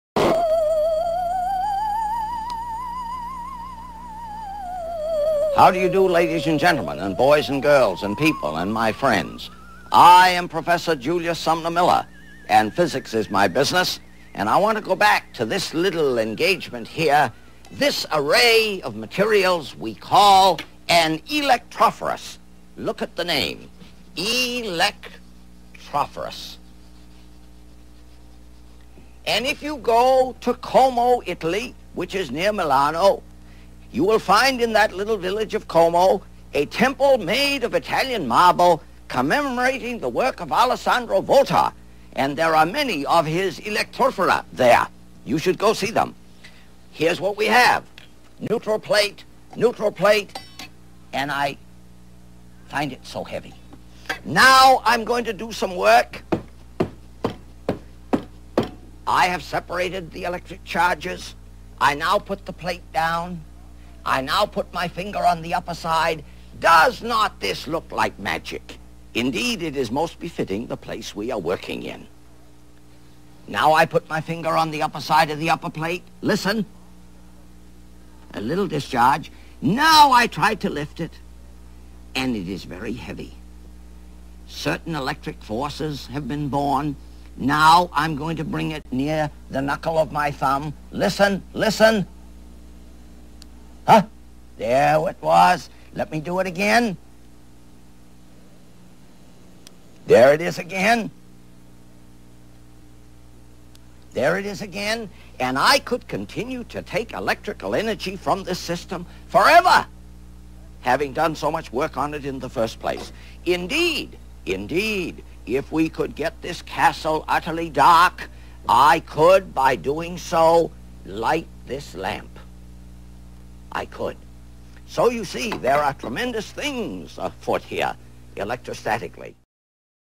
Julius Sumner Miller 46 HHOF - Electrostatics Cont.
Professor Julius Sumner Miller demonstrates an electrophorous.  The Professor hits a plastic plate with some fur which transfers some of the electrons from the fur onto the plastic plate, giving the plastic plate a negative charge.  The Professor then places a metal plate with an insulated handle (plastic) onto the plastic plate.  The negatively charged plastic plate repels the electrons within the metal plate, giving the metal plate a polar charge.  The metal plate becomes negatively charged on the top, and positively charged on the bottom.  The Professor grounds the metal plate by placing his finger on the top of the metal plate.  The electrons on the top of the metal plate now becomes stored in/on The Professor.  The Professor then lifts the metal plate, thus doing work on the metal plate, this work is transferred into separating the charges on the plate again into positive (bottom) and negative (top); however, this charge separation results in a DIFFERENCE in the electric potential between the top and bottom of the metal plate.  This creates a voltage, which The Professor demonstrates by getting an electric shock when placing his knuckle next to the metal plate.  As stated by The Professor, this process can be repeated by placing the metal plate back onto the plastic plate, grounding the metal plate, and then raising the plate again.